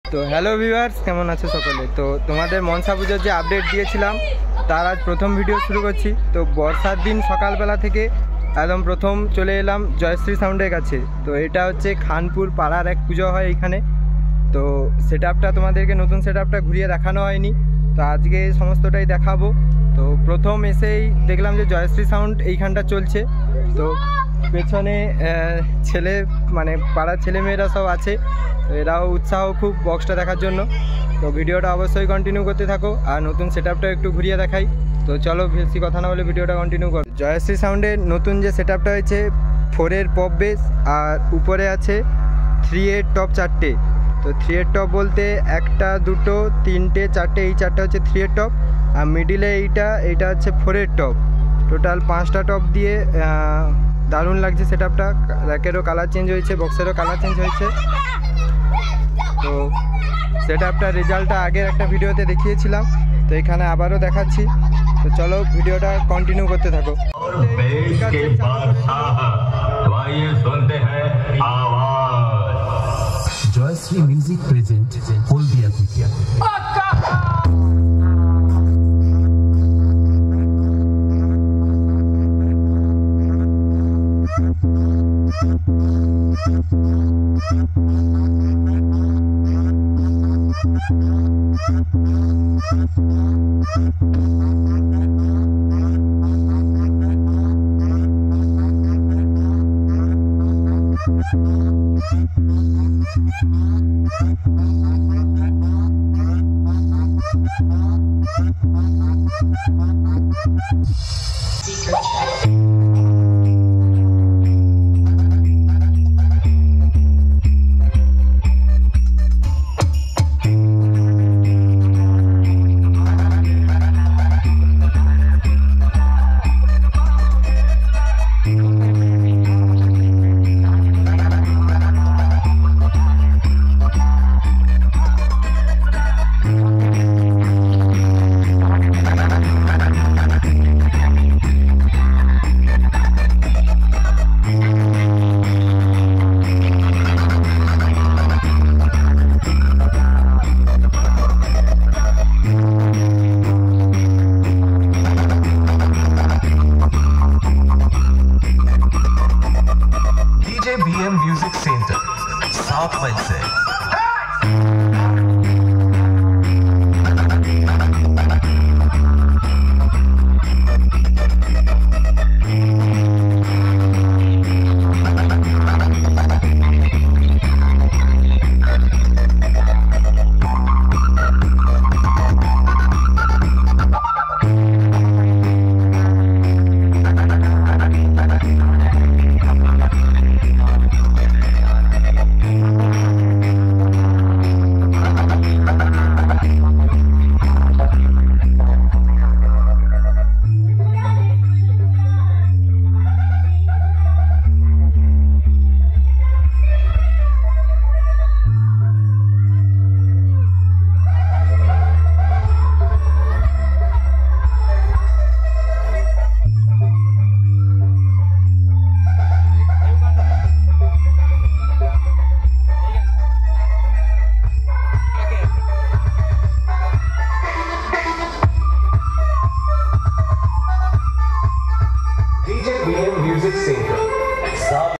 3top+4top 5টা সাথে 2000watt sub(c) নিয়ে দারুণ রেজাল্ট করলো Jayshree Sound 🌟একাই খাঁনপুর কাঁপাচ্ছে🌟
3top+4top 5টা সাথে 2000watt sub(c) নিয়ে দারুণ রেজাল্ট করলো Jayshree Sound 🌟একাই খাঁনপুর কাঁপাচ্ছে🌟